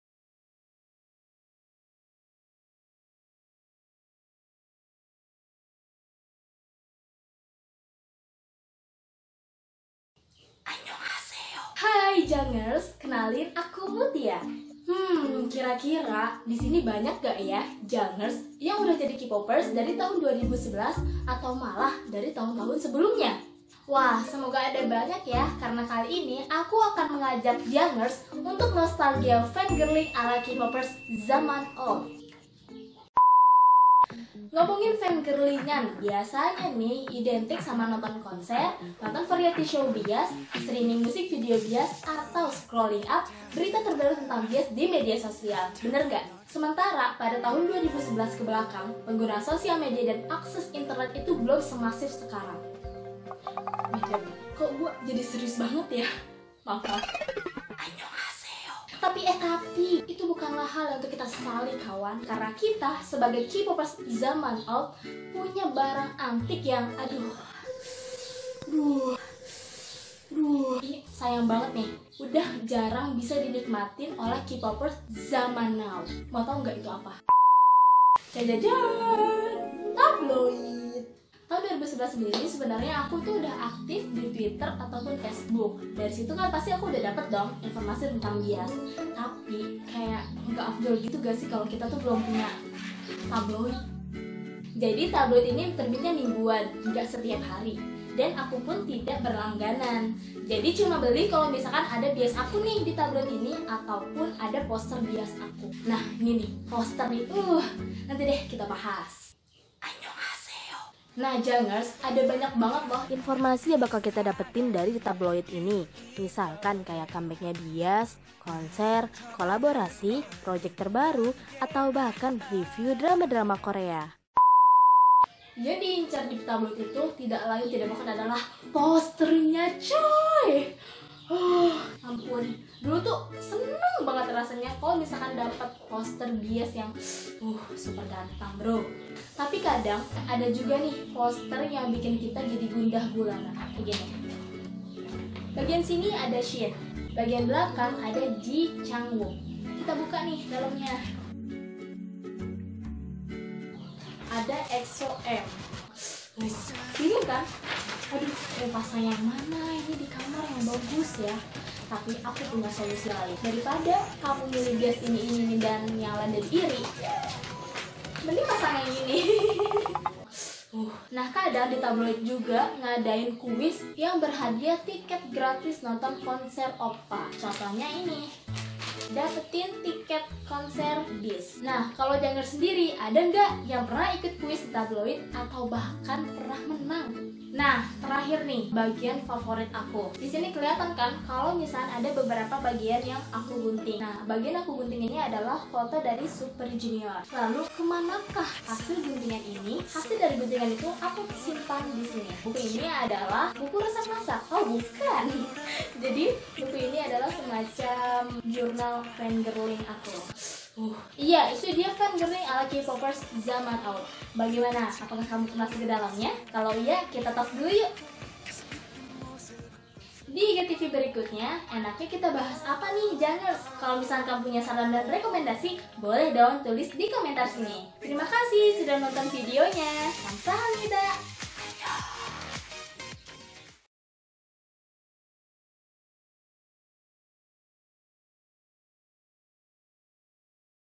[KUTALK 1] Fangirling Ala K-Popers Zaman Old
Hallo Jjangers!
Kamu Generasi K-Popers Zaman Old atau Zaman Now?
untuk kamu K-Popers Zaman Old, yuk nostalgia keseruan fangirling ala Kpopers Zaman Old! Dan untuk kamu K-Popers Zaman Now, yuk intip keseruannya!